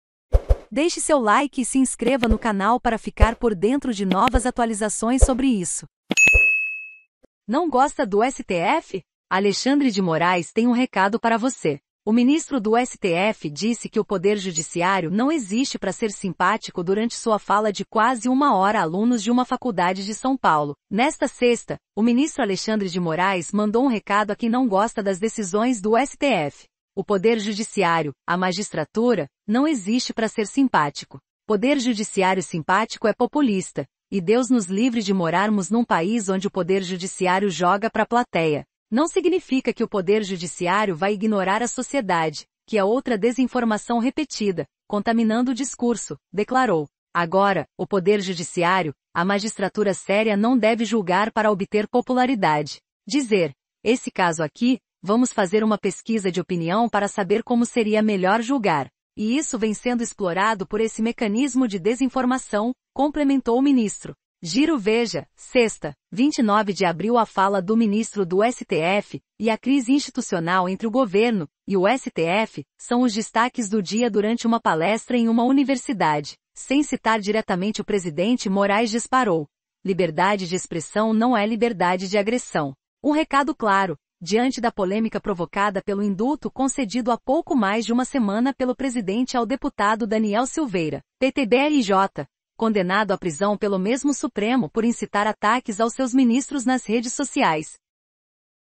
NÃO GOSTA DO STF? ALEXANDRE DE MORAES TEM UM RECADO PARA VOCÊ
O ministro do STF disse que o Poder Judiciário não existe pra ser simpático Durante sua fala de quase uma hora a alunos de uma faculdade de São Paulo. nesta sexta. o ministro Alexandre de Moraes mandou um recado a quem não gosta das decisões do STF. “O Poder Judiciário. a magistratura. não existe pra ser simpático. Poder Judiciário simpático é populista. e Deus nos livre de morarmos num país onde o Poder Judiciário joga pra plateia. Não significa que o Poder Judiciário vai ignorar a sociedade. que é outra desinformação repetida. contaminando o discurso”. declarou. “Agora. o Poder Judiciário. a magistratura séria não deve julgar para obter popularidade. [Dizer:] ‘Esse caso aqui. vamos fazer uma pesquisa de opinião para saber como seria melhor julgar’. E isso vem sendo explorado por esse mecanismo de desinformação”. complementou o ministro. Giro VEJA   sexta. 29 de abril A fala do ministro do STF e a crise institucional entre o governo e o STF são os destaques do dia Durante uma palestra em uma universidade. sem citar diretamente o presidente. Moraes disparou: “Liberdade de expressão não é liberdade de agressão”. Um recado claro. diante da polêmica provocada pelo indulto concedido há pouco mais de uma semana pelo presidente ao deputado Daniel Silveira (PTB RJ). condenado à prisão pelo mesmo Supremo por incitar ataques aos seus ministros nas redes sociais